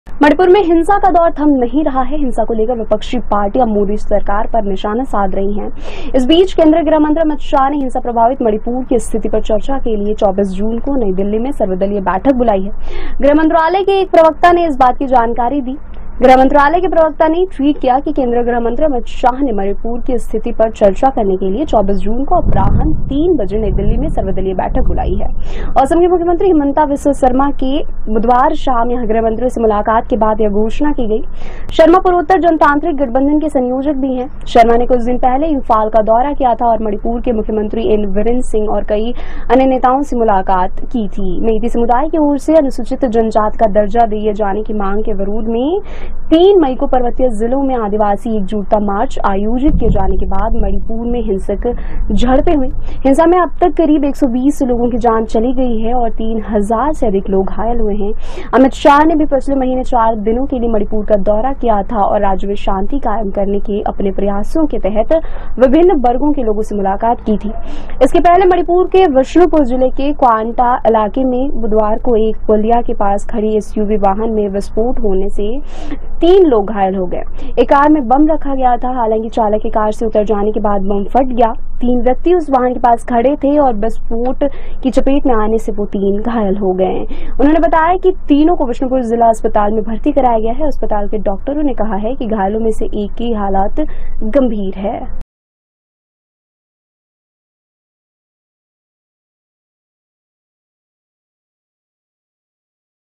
Manipur Violence: थम नहीं रही हिंसा, SUV में बम धमाके से 3 घायल, अमित शाह ने 24 जून को बुलाई बैठक
मणिपुर में हिंसा का दौर थम नहीं रहा है। हिंसा को लेकर विपक्षी पार्टियां मोदी सरकार पर निशाना साध रही हैं। इस बीच केंद्रीय गृह मंत्री अमित शाह ने हिंसा प्रभावित मणिपुर की स्थिति पर चर्चा के लिए 24 जून को नई दिल्ली में सर्वदलीय बैठक बुलाई है। गृह मंत्रालय के एक प्रवक्ता ने इस बात की  जानकारी दी।